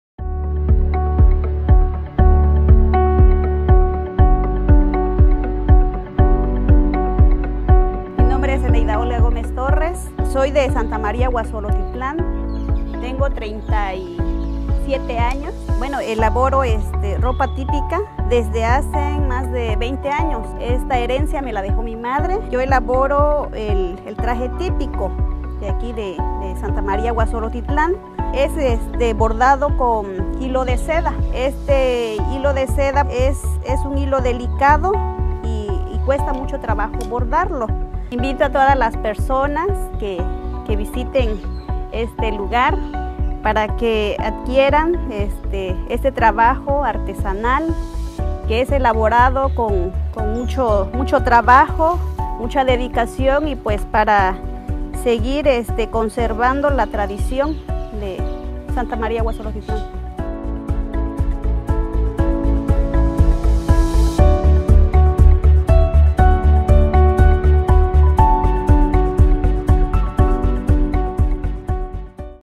Bordado Huazolotitlan
Huazolotitlan
Significa "Lugar de Guajolotes", etimología "Huexlotl": guajolote y "Titlán": lugar de ) es uno de los  municipios pertenecientes al distrito de Santiago Jamiltepec, Oaxaca.
En esta localidad se elabora ropa típica con bordados a mano en diferentes colores y dibujos propios de la región.